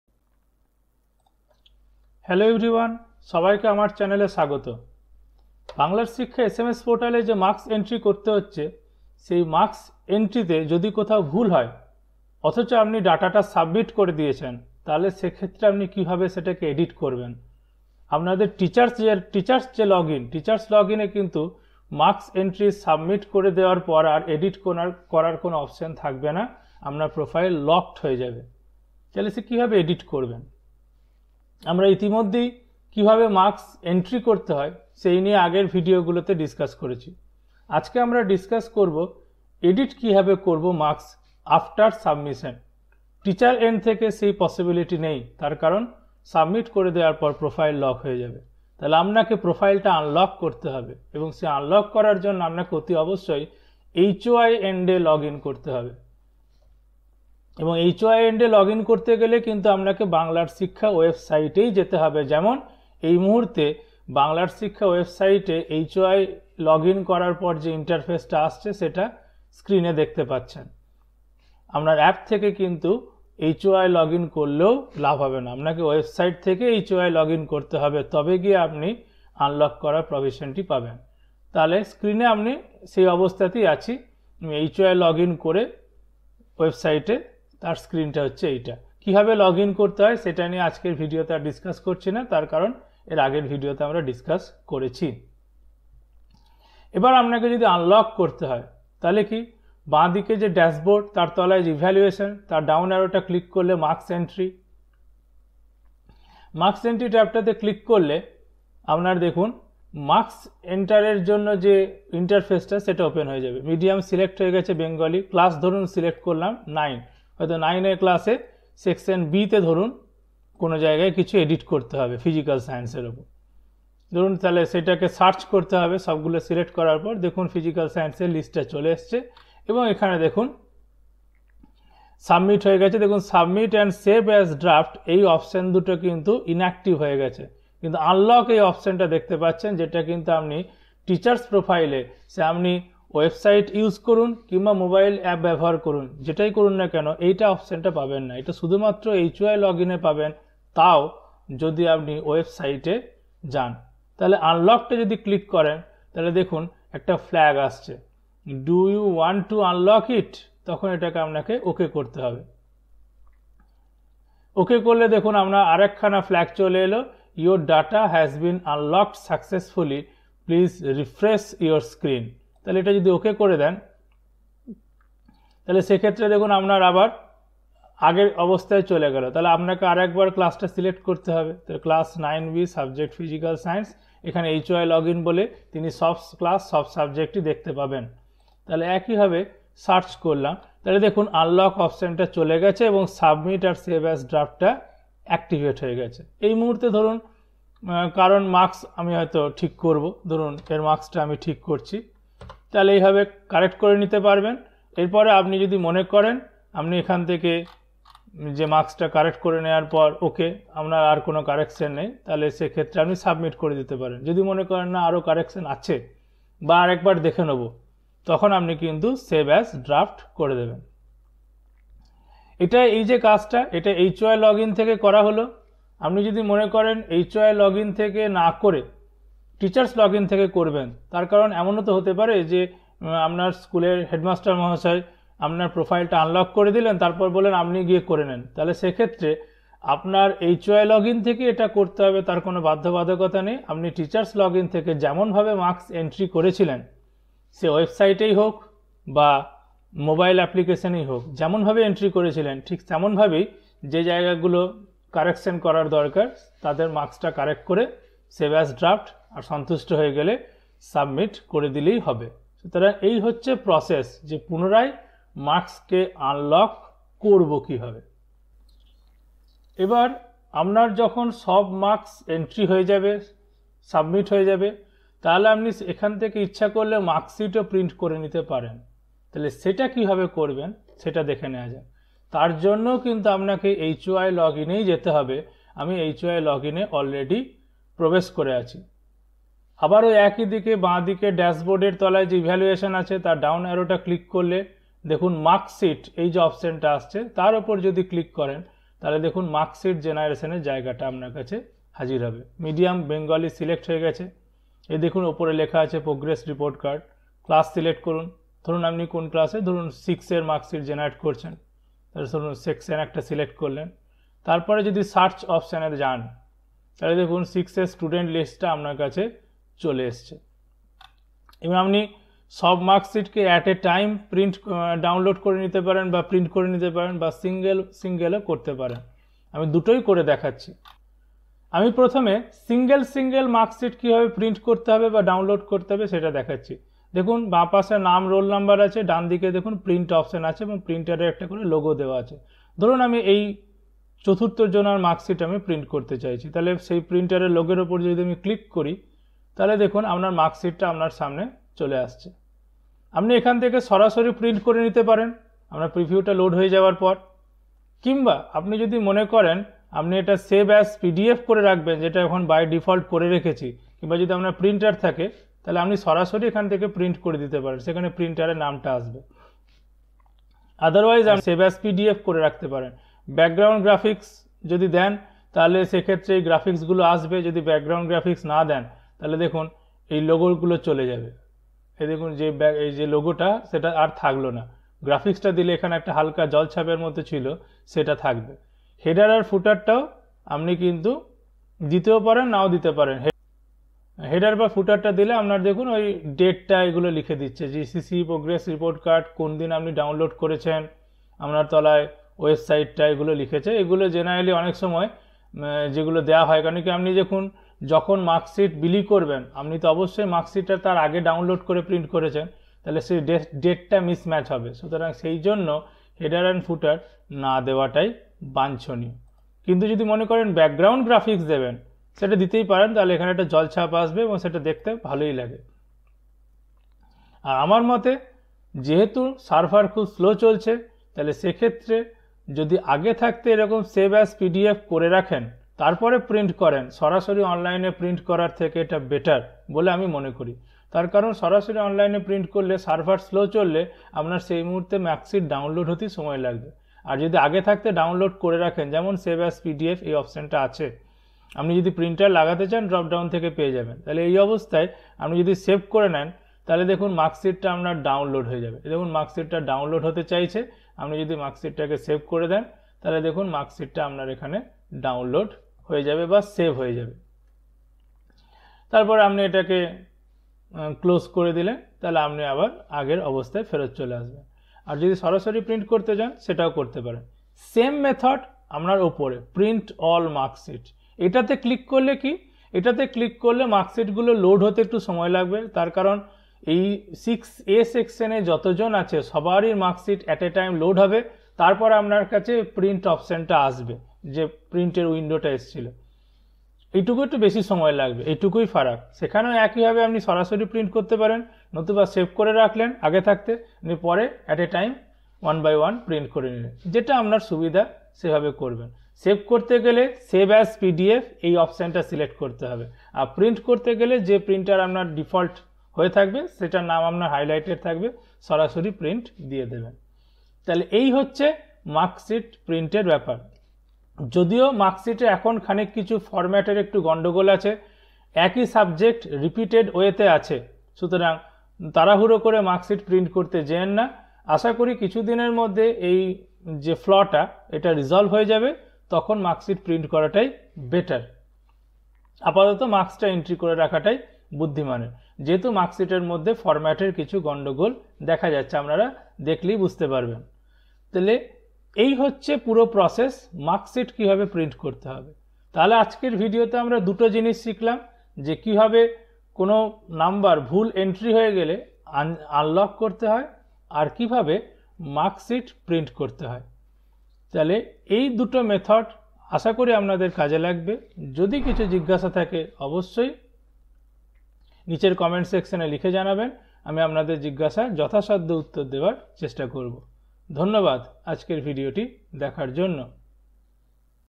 Marks entry profile unlock in SMS Portal ।। Marksheet download
এখন প্রত্যেক শিক্ষক-শিক্ষিকাদের একটি অন্যতম গুরুত্বপূর্ণ কাজ হল Final Model Activity Task এর Evaluation করে ছাত্র-ছাত্রীদের প্রাপ্ত নম্বর বাংলার শিক্ষা পোর্টালে Upload করা। এখন Banglar Shiksha portal marks upload ভুল হয়ে গেছে অথচ তিনি Marks Submit করে দিয়েছেন ! সেক্ষেত্রে তিনি কী করবেন, কীভাবে Marks entry profile unlock in SMS Portal করা যায়—সে সব এই ভিডিওতে বিস্তারিত আলোচনা করা হয়েছে। এছাড়াও কীভাবে Banglar Shiksha sms portal হতে CCE Progress Report generate করবেন বা Marksheet download করবেন, সেটাও দেখানো হয়েছে এই ভিডিওতে।